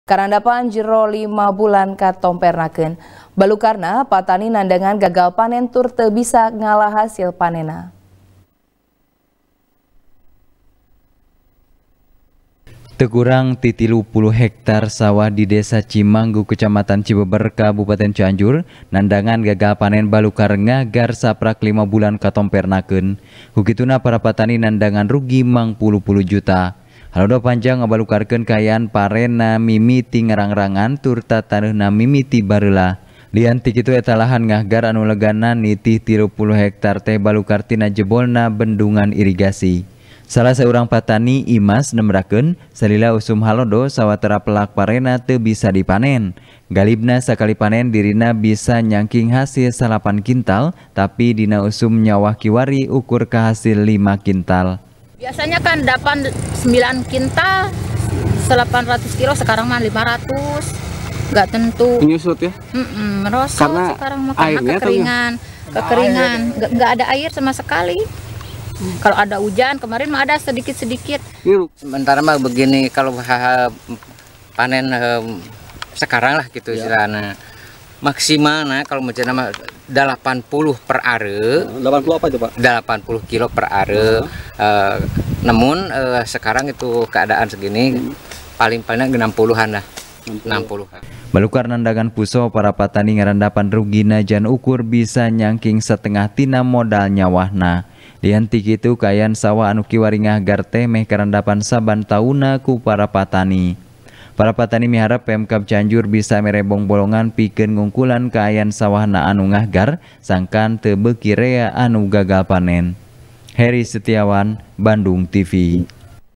Halodo Panjang, Sawah Mangpuluh-Puluh Hektar Puso, Patani Rugi | TP | BANDUNGTV
TP | BANDUNGTV | ELBY NOPITRI | Usum Halodo Di Kabupaten Cianjur, Anu Ngabalukarkeun Mangpuluh-Puluh Hektar Sawah Patani Ngahgar, Geus Karandapan Jero Lima Bulan Katompernakeun. Balukarna Para Patani Nandangan Gagal Panen Tur Teu Bisa Ngala Hasil Panenna .